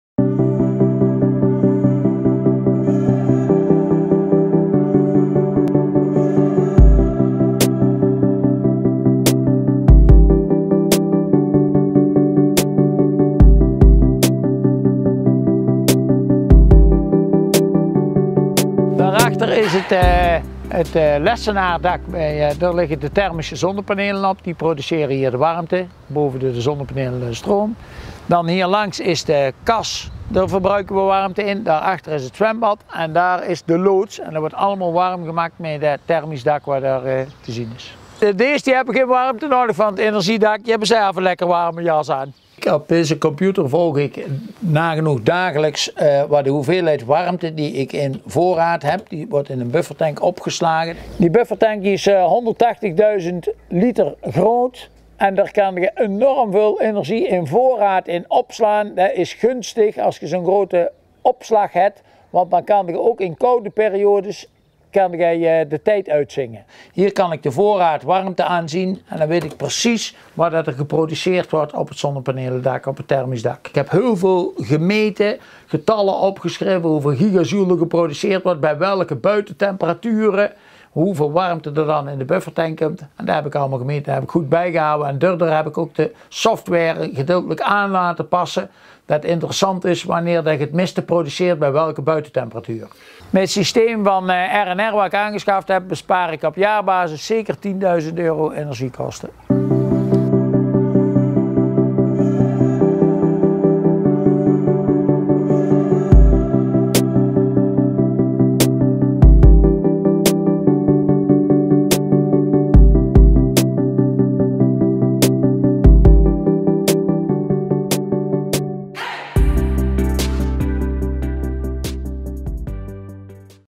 Energieverdieners - Toon Kanters
(Oud) ondernemer Toon Kanters mag nog graag een beetje 'boeren' in zijn 'vrije tijd'.
Voor zijn energievoorziening maakt hij gebruik van de Power Profit zonnepanelen van R&R Systems. Toon bespaart met het systeem op jaarbasis zeker €10.000 op zijn energiekosten.
Ons deskundig team wil ook voor u haar kennis inzetten voor een duurzame energieoplossing.